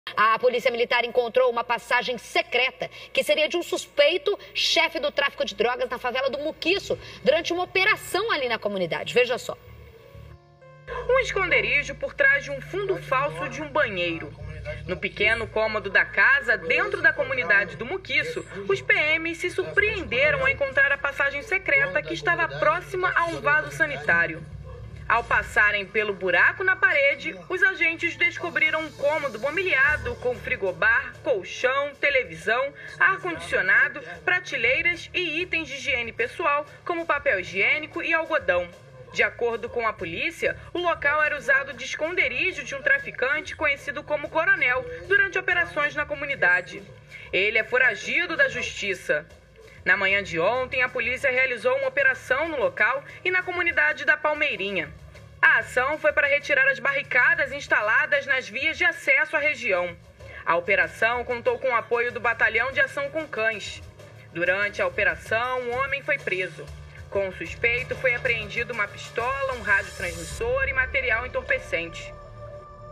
Polícia militar encontra passagem secreta de suspeito de chefe de tráfico de drogas no Muquiço
A polícia militar encontrou uma passagem secreta, que seria de um suspeito chefe do tráfico de drogas da favela do Muquiço, durante uma operação na comunidade.